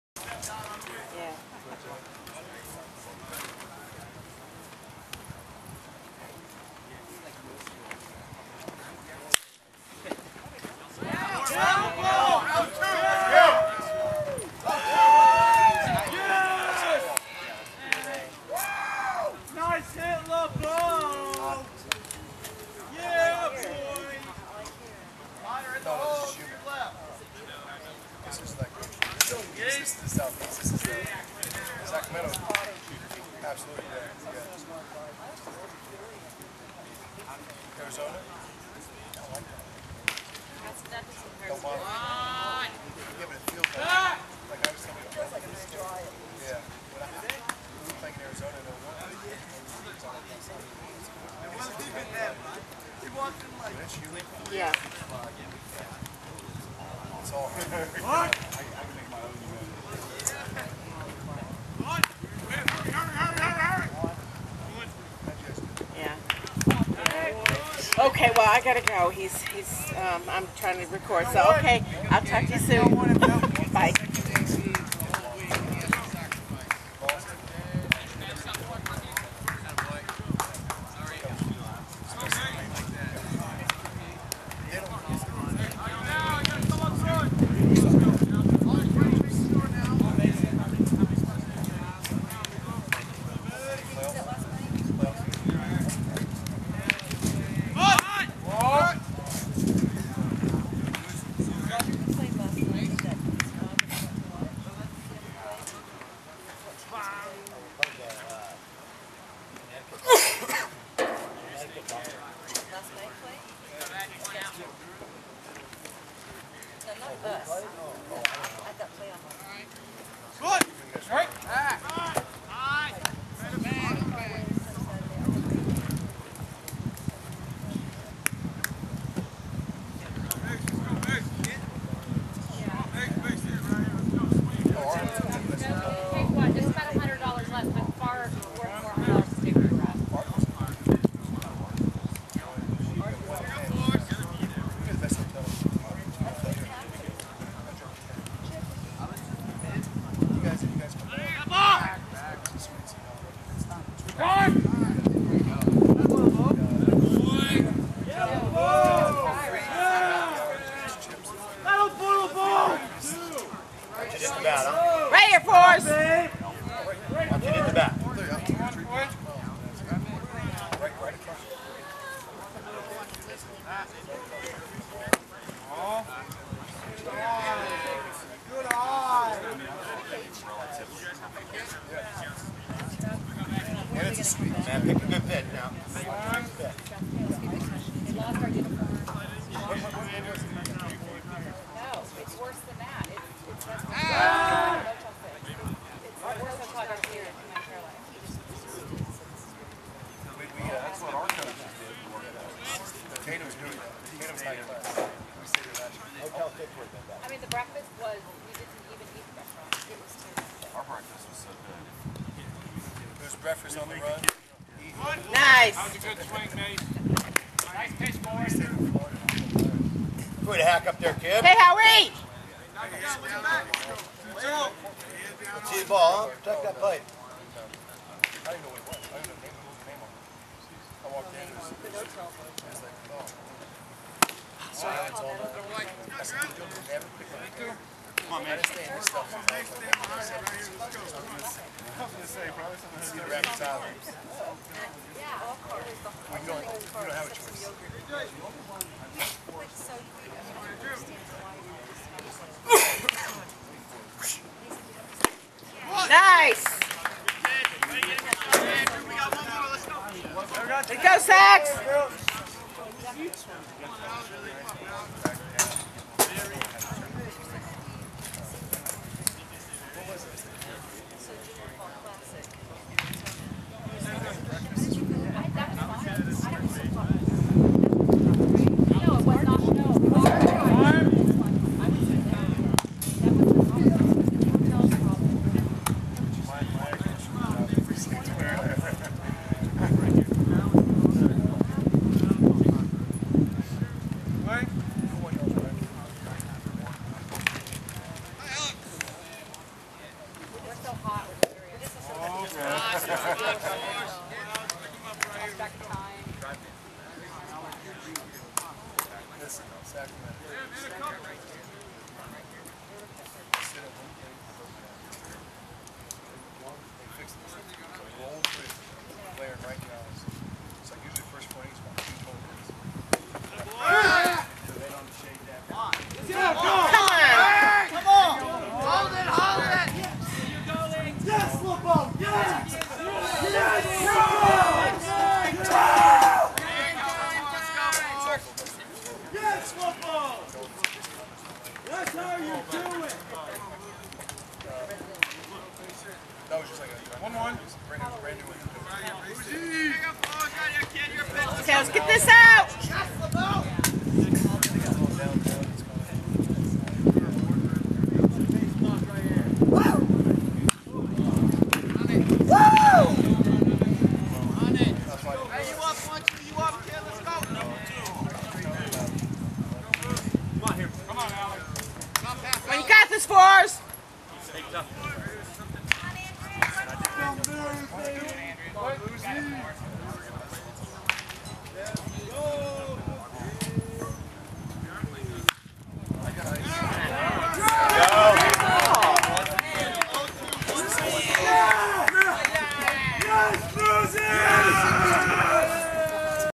7/14/11 EC RED SOX 44 ANDREW HOWARD 6th IP
Baseball East Cobb Red Sox #44 Andrew Howard Pitching  vs EDH Vipers in 17U Perfect Game WWBA. 2/3 inning pitched. BF 4, PC 12, 1HE, 1SO, 1GO, 1S. Strikes 8, Balls 4. 1 unearned run. Final score 0-1. ER0, SO 4, FO 5, GO 8, PO 1, BB 0, Errors By Pitcher 0.